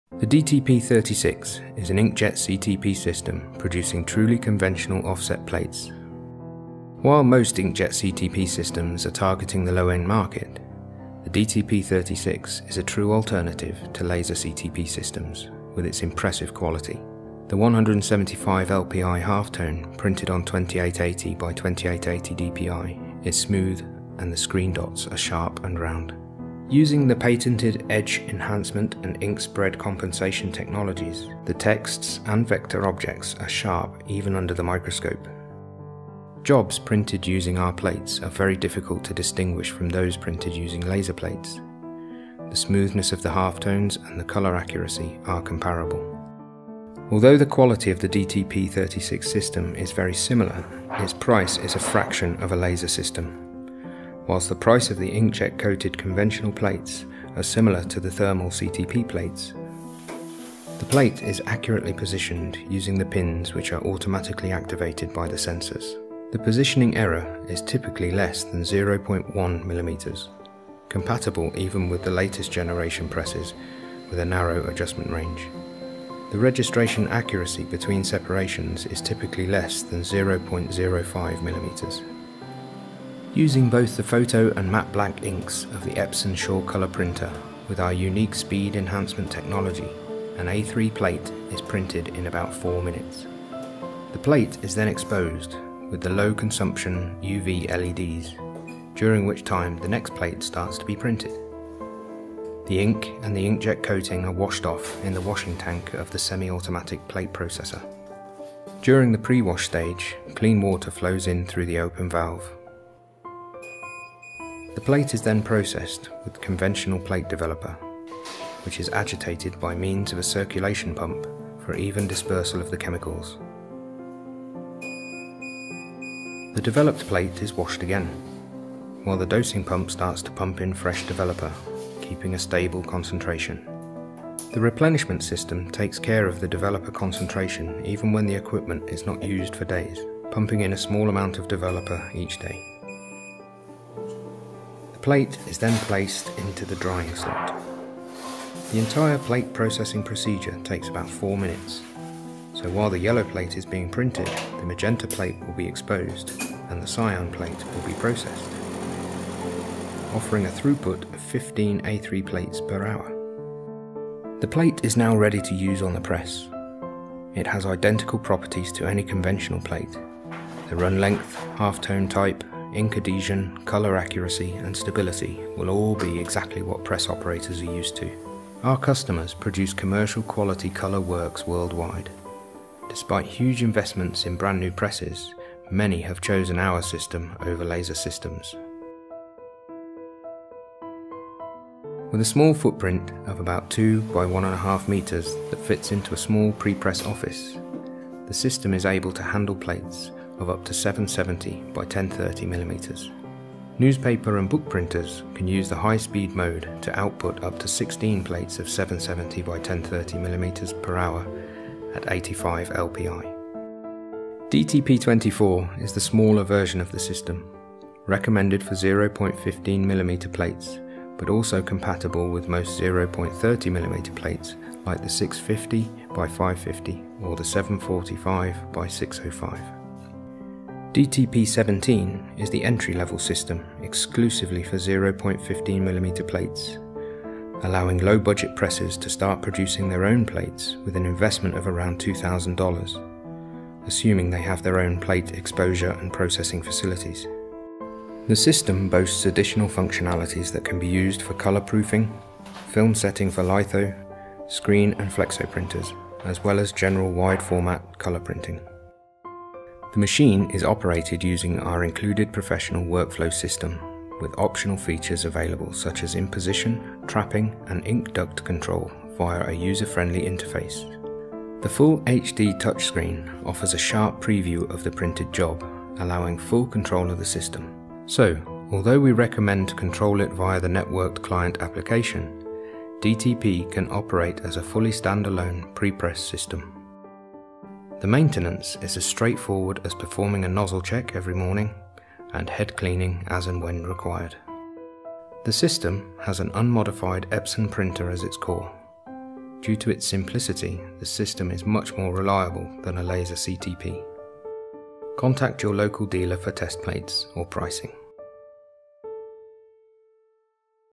DTP 36, 24 and 17 presentation
A presentation of the facelifted 2018 models of the StudioRIP Droplet to Plate 36, 24 and 17 inkjet CTP systems.